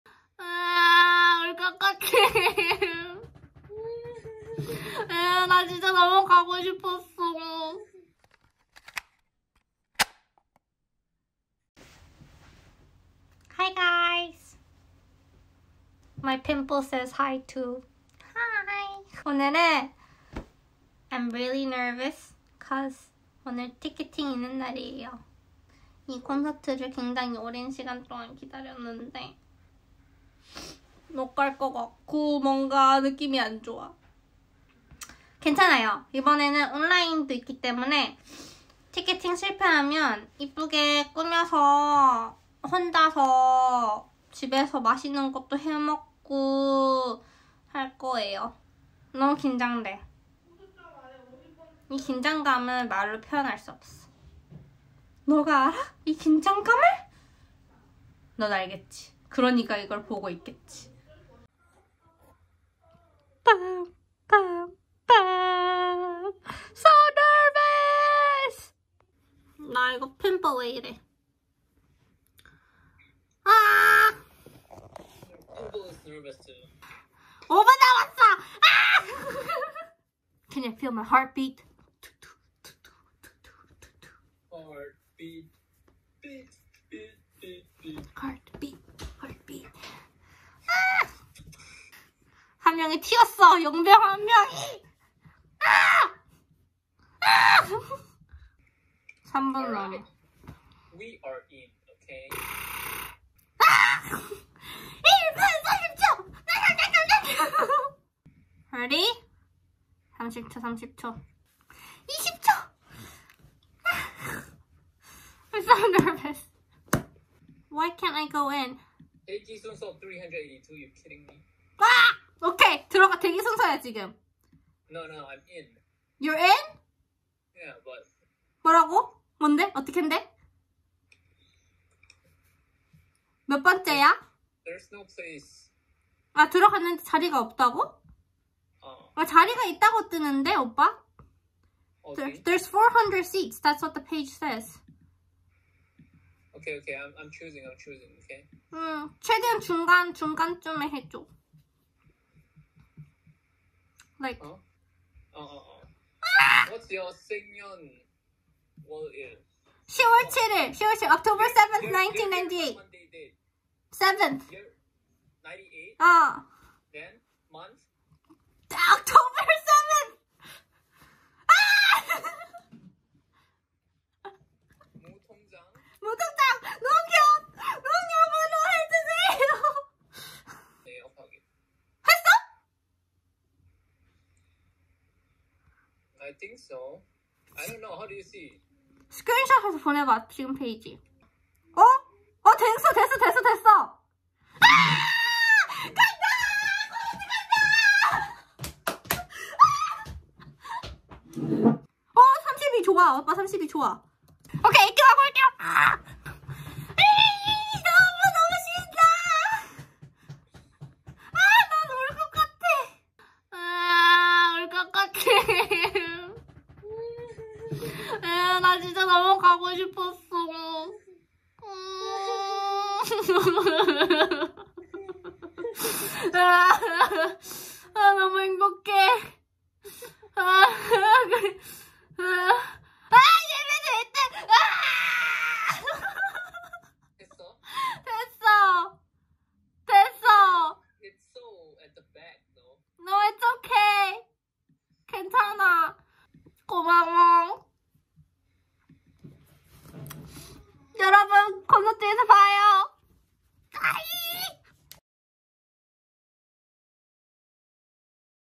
kpop concert ticketing reality n년차 멜로디의 비투비 10주년 콘서트 오프라인 티켓팅 (feat. BTOB and Melody)
티켓팅은 매번 떨리는 것. 저는 첫콘 32구역 이에요!! 만약에 보게 된다면 보면 인사해주세용 Ticketing always makes me nervous... I am sitting at section 32 on the first day of the concert!! Don't be shy to say hi when you see me!!

+ 콘서트 브이로그에서 보고 싶은 것들이 있다면 알려주세요!! 제가 최선을 다해서 정성 담아 담아오겠습니다 (콘서트 무대는 당연히 안 되는 거 아시죠 :) Let me know what you want to see in the concert vlog!! I will do my very best to get every single detail!! (of course not the concert itself)